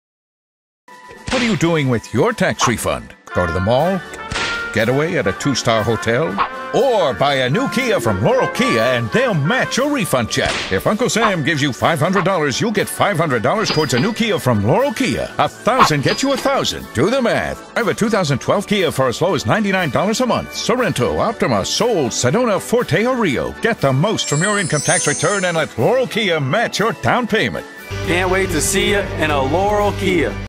Laurel Kia's Newest Commercial
Laurel Kia will match your tax refund check